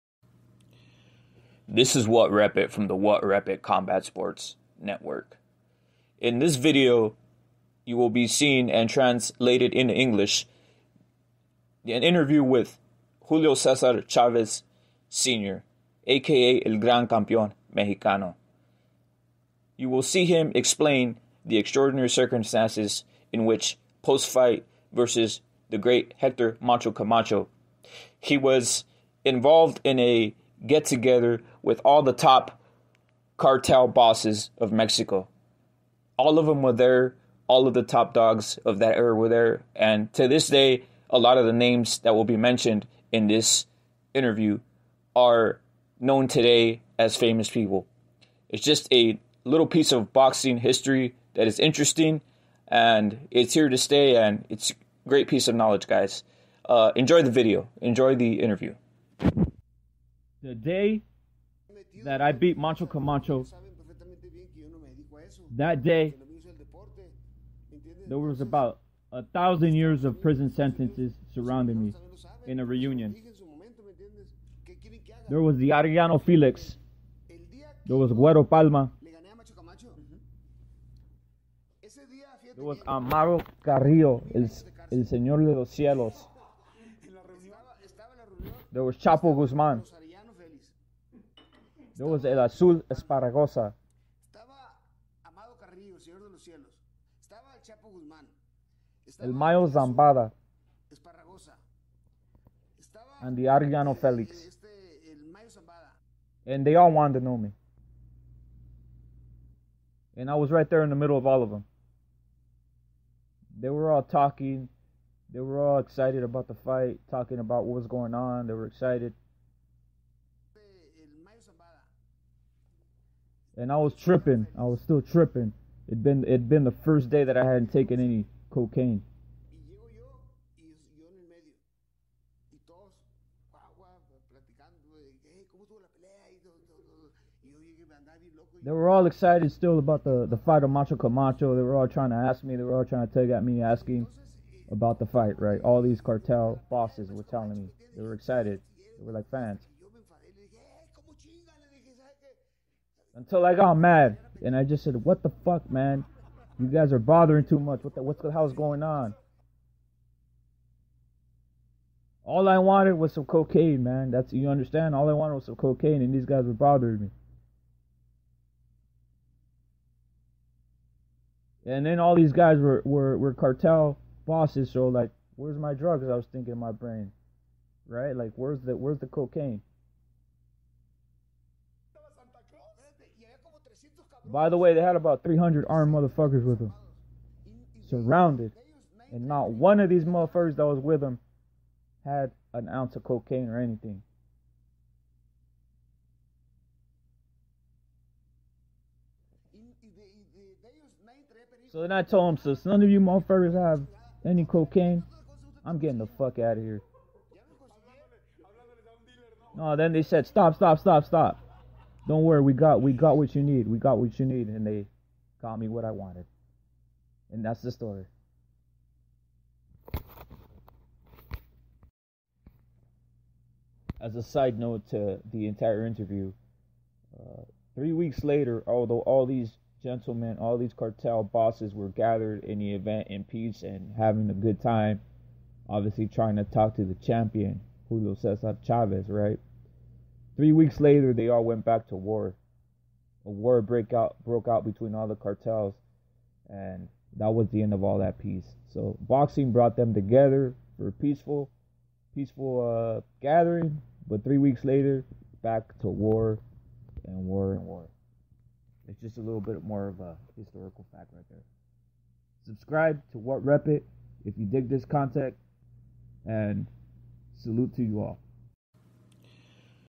Julio Cesar Chavez & Mexican Cartel King Pins . Epic 1992 post fight gathering after beating Camacho
Translated by whatREPit - In this video Julio Cesar Chavez reveals details of his legendary post fight  1992 win vs Macho Camacho. Revealing a gathering/party involving all the top Kingpins of every major Mexican Cartel.